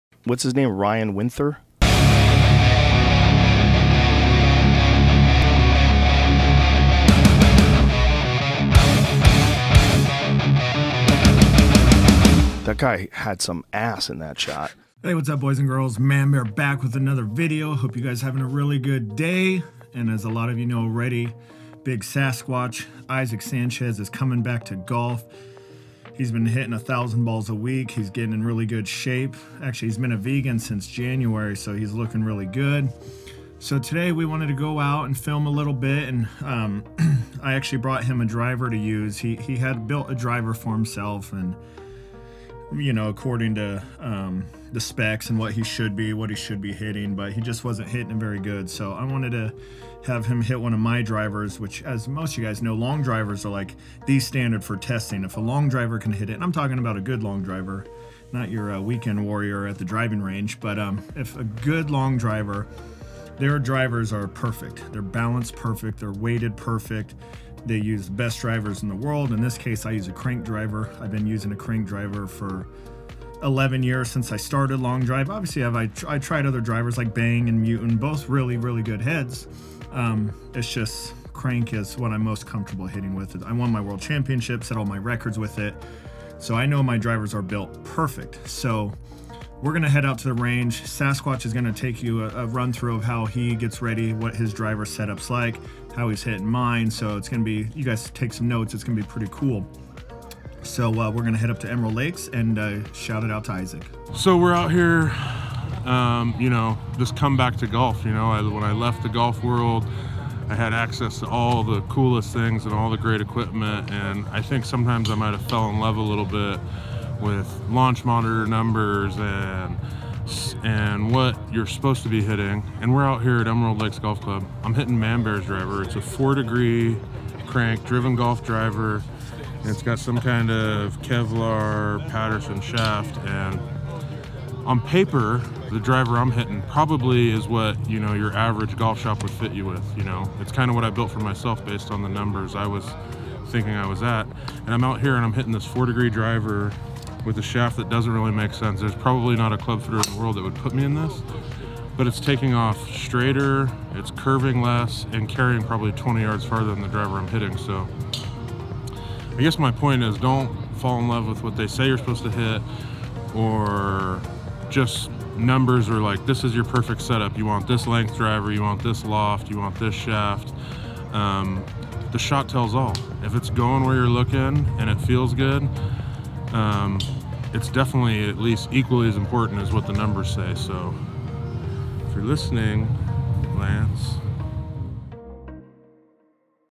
Sasquatch hits my driver!
With Sasquatch's help, it's important to realize that just because a club fitter fits you "perfectly," it doesn't mean it's the right setup for you.  The most important thing is that you find a driver that you can hit straight, long and consistent..  Sasquatch went through a fitting, got dialed in, and now the driver doesn't go straight. So he started using my 4 degree, 47 inch 3x shaft driver, and guess what?! He hits it damn near perfect each and every time.  Obviously him being the quality of player he is, it isn't typical for everyone, but it shows you that the most important thing is finding the driver that you can hit straight!